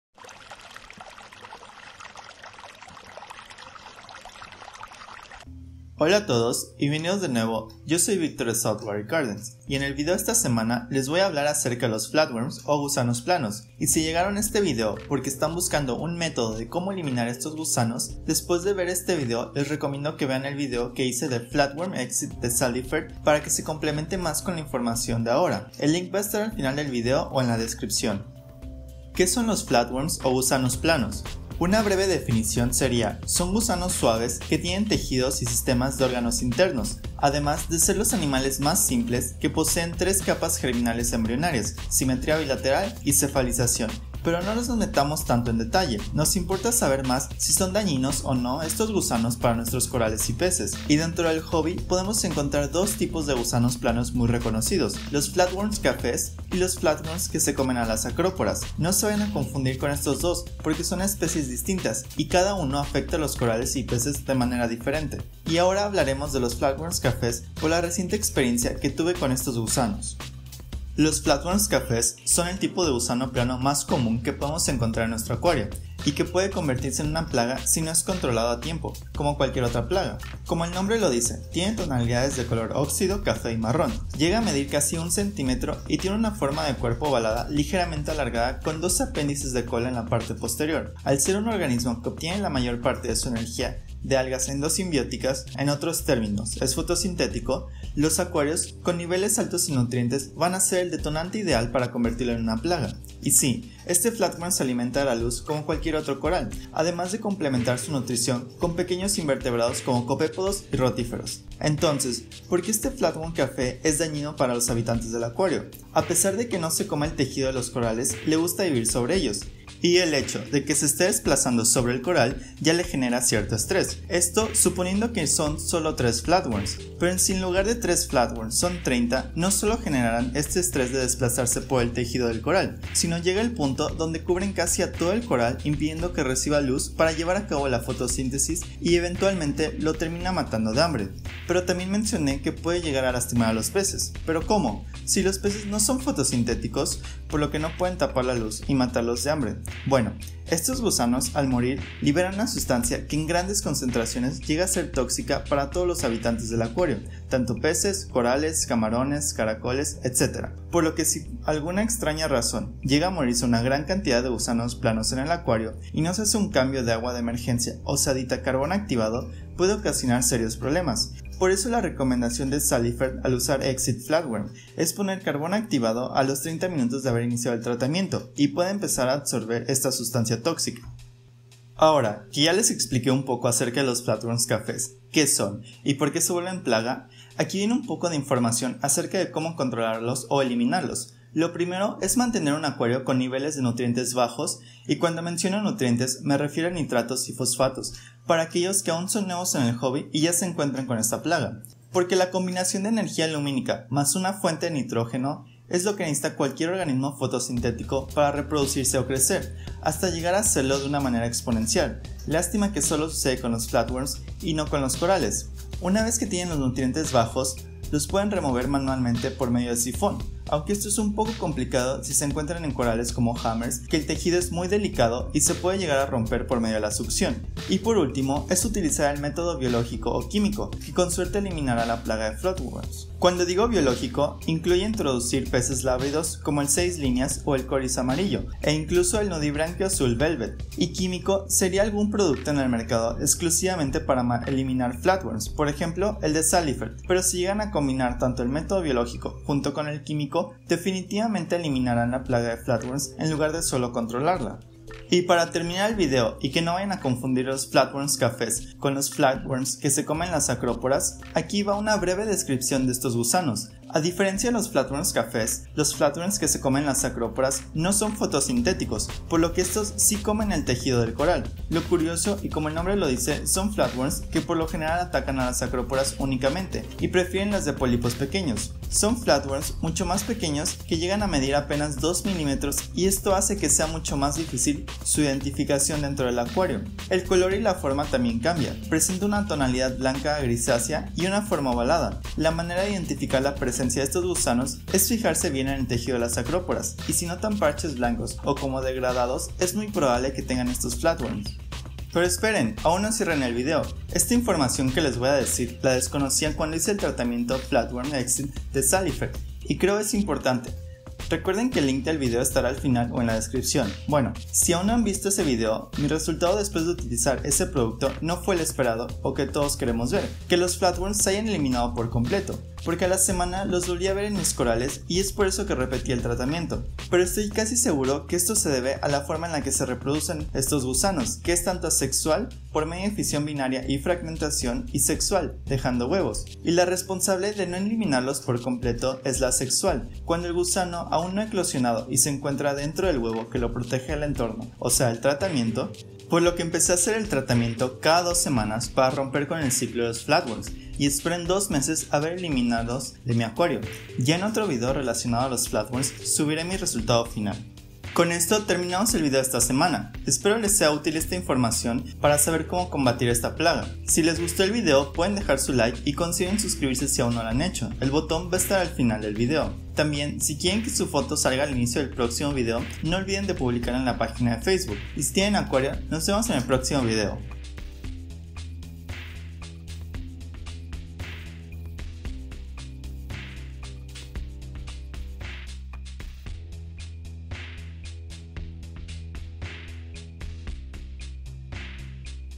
¿Qué Son Los Flatworms?
En el video anterior les conte mi experiencia utilizando el producto eXit Flatwroms de la marca Salifert, ahora le cuento un poco acerca de los gusano planos o "flatworms" en el acuario.